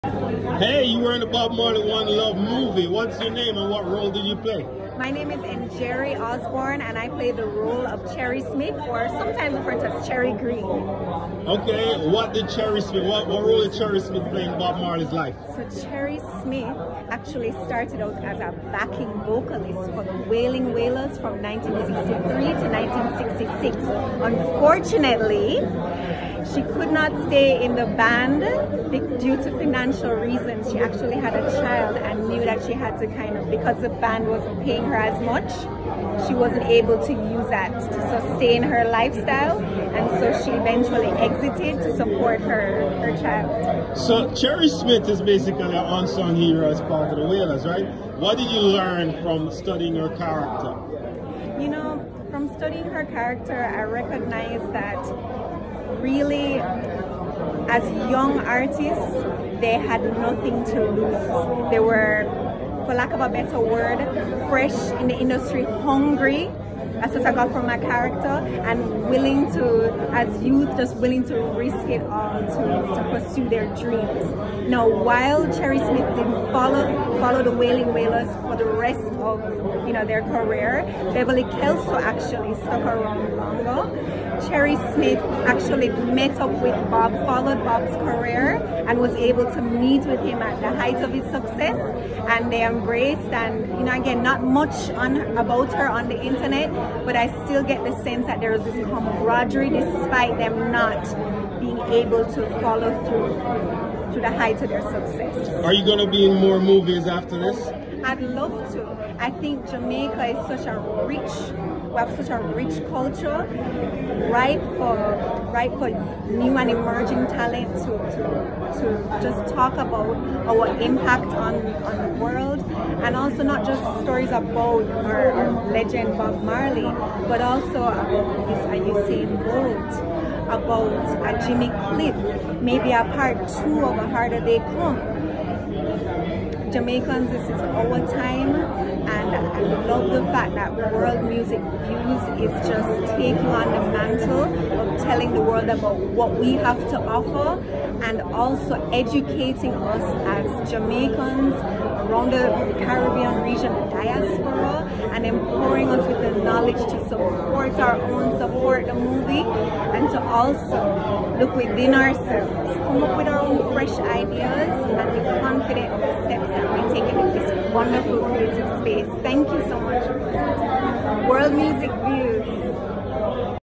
Cherry Green (Actor) From Bob Marley: One Love Movie interview
Bob Marley: One Love movie actor speaks with World Music Views about her role in the Paramount pictures film